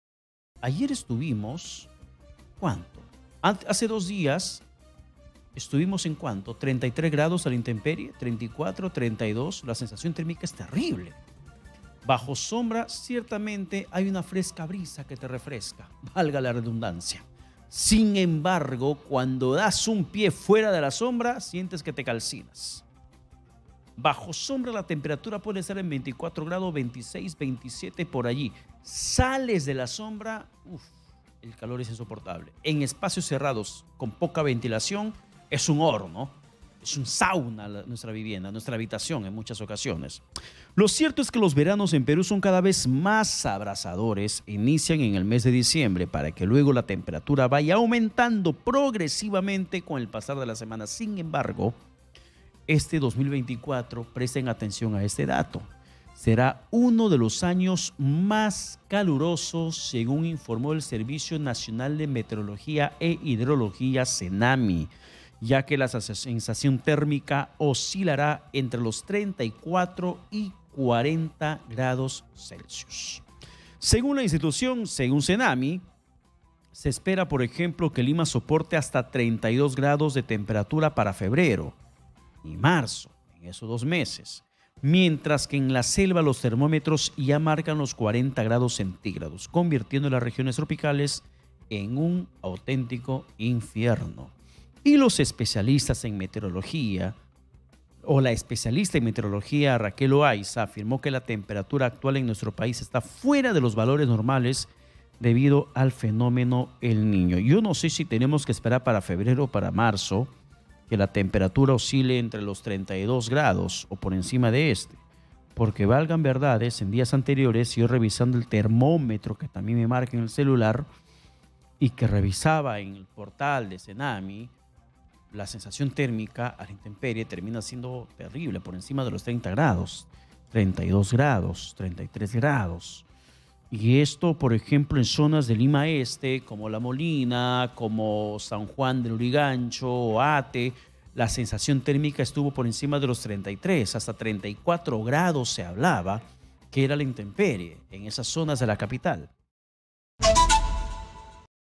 🔴🔵Altas temperaturas provocarán sensación térmica entre los 34 y 40 grados
Los veranos en Perú son cada vez más abrasadores e inician en el mes de diciembre, para que luego la temperatura vaya aumentando progresivamente con el pasar de las semanas. Sin embargo, este 2024 será uno de años más calurosos según indicó Senamhi, ya que la sensación térmica oscilará entre los 34 y 40 grados celsius.

Exitosa Perú
"Exitosa: La voz que integra al Perú"

Corporación Universal